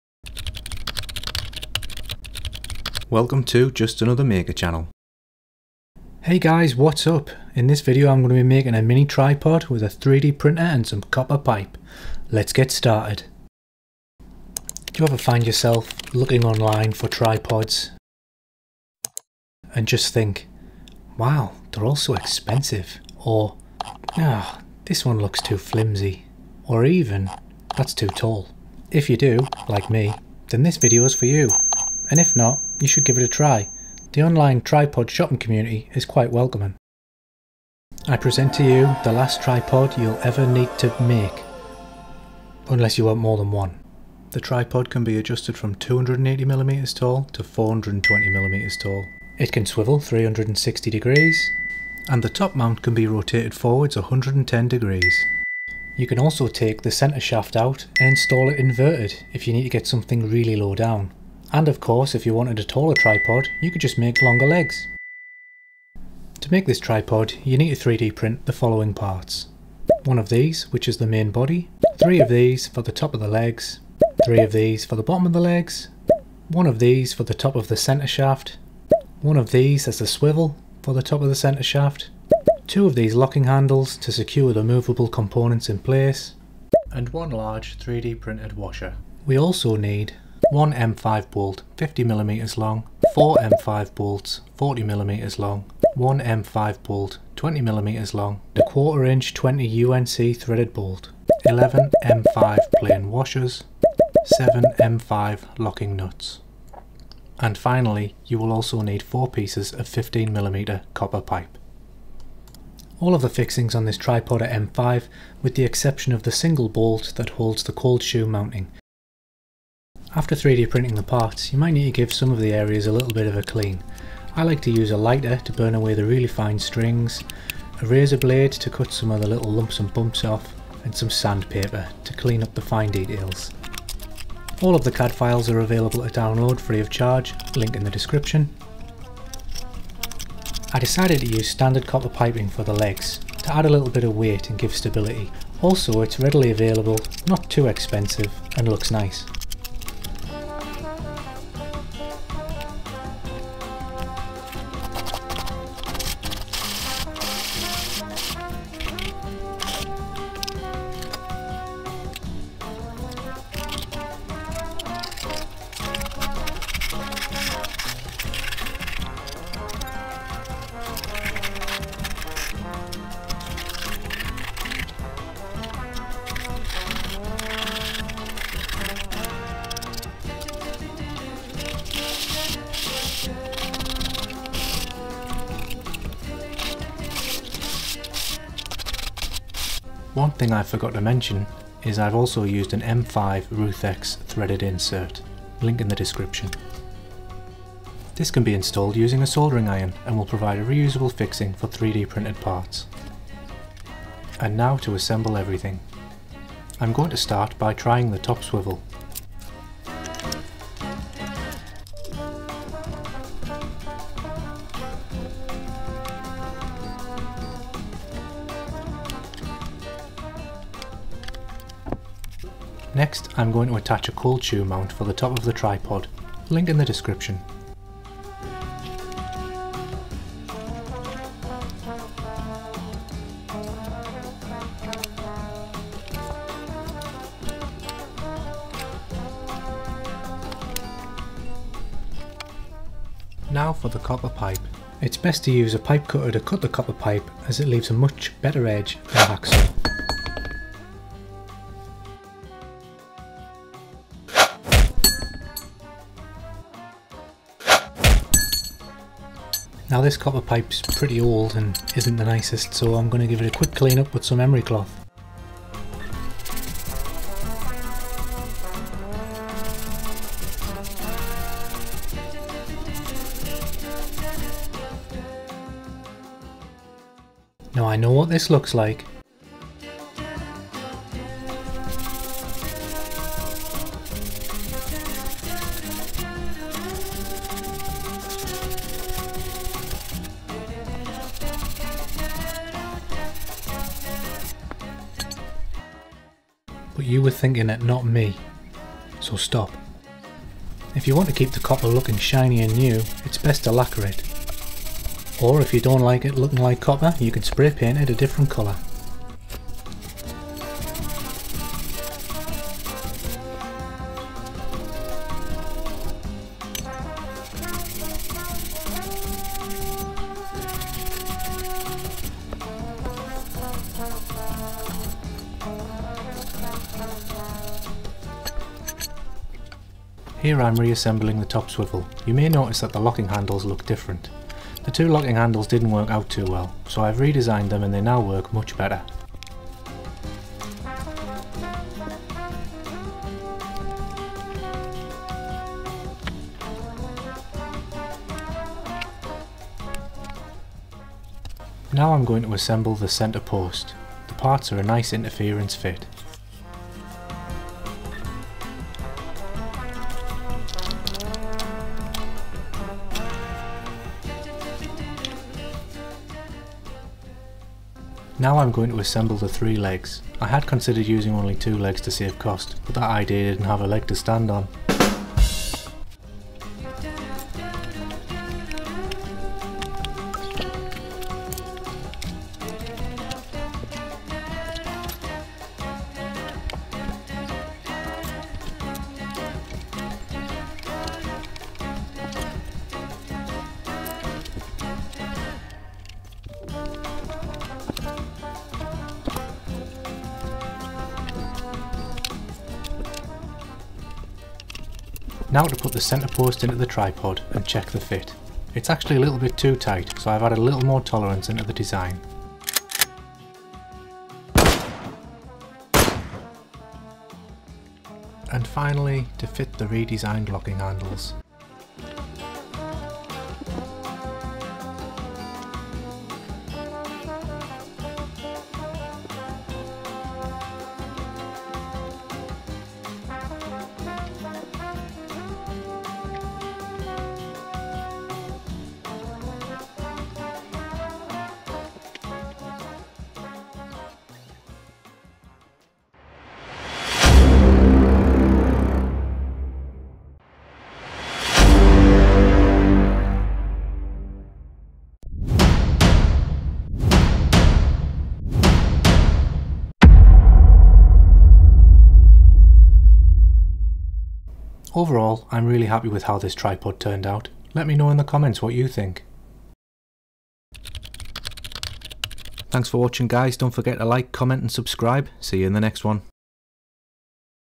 3D Printed Mini Tripod
In this video I'm going to be making a mini Tripod with my 3D printer and some copper piping.

Cold Shoe Mount
Threaded Inserts (They have gone up in price, there are cheaper alternatives)